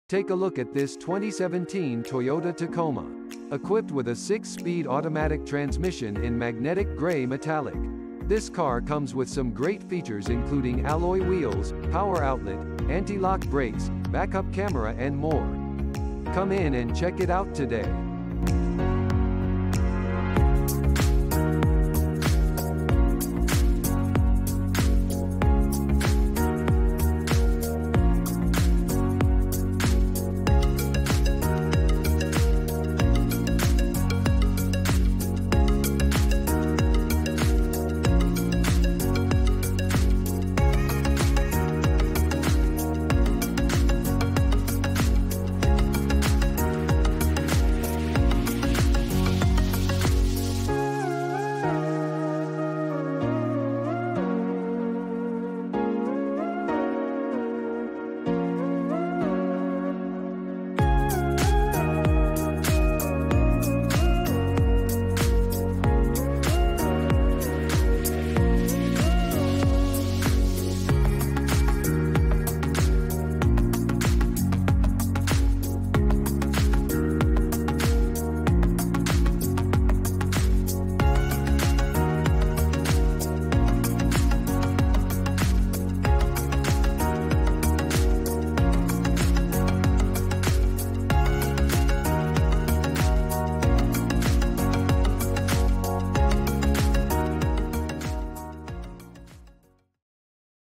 2017 Toyota Tacoma TRD Off-Road UP094950
Take a look at this 2017 Toyota Tacoma! Equipped with a 6-Speed Automatic transmission in Magnetic Gray Metallic. This car comes with some great features including Alloy Wheels, Power Outlet, Antilock Brakes, Backup Camera and more. Come in and check it out today!